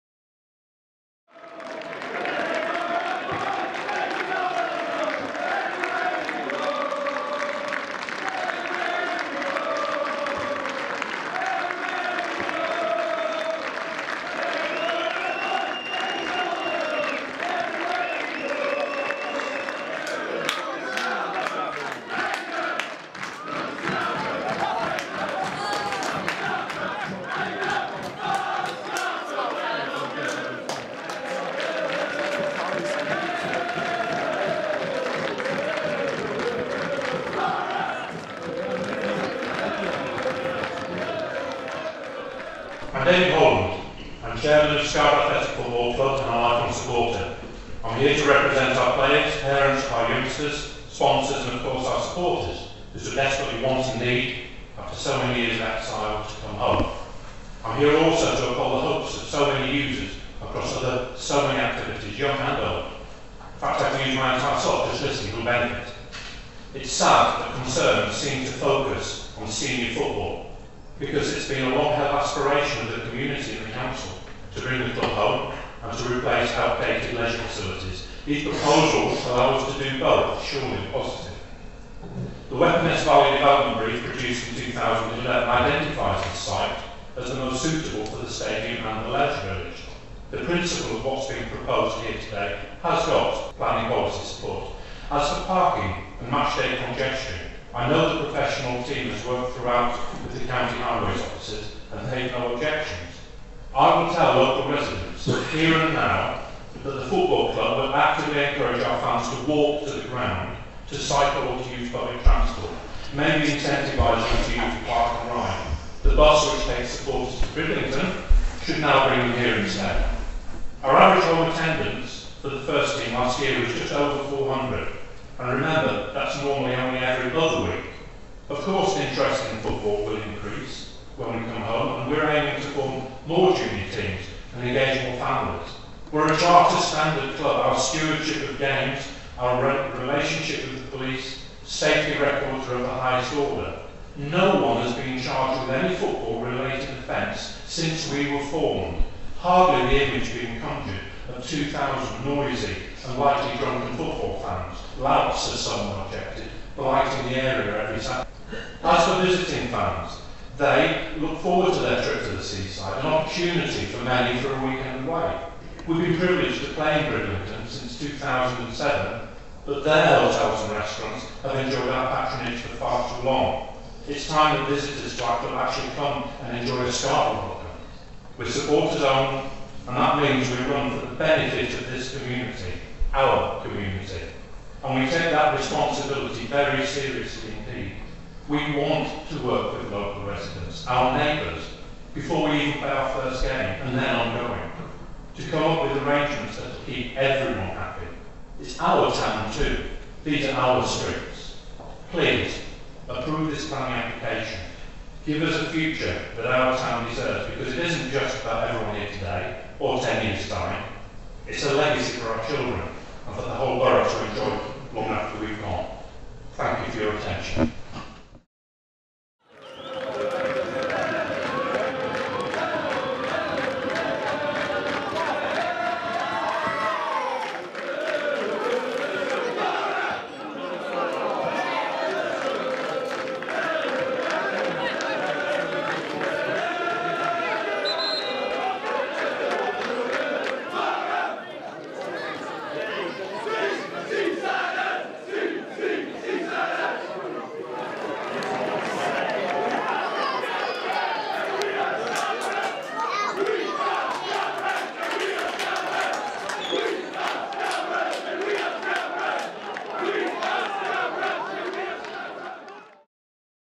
"I'm David Holland"
The late Chairman of Scarborough Athletic Football Club, David Holland, presenting to the Scarborough Borough Council Planning and Development Committee in 2015 on behalf of the applicants for the Scarborough Sports Village development.

This video was produced to commemorate the first anniversary of David passing away and to reflect on the return of football to Scarborough, for which so much is owed to the efforts of David.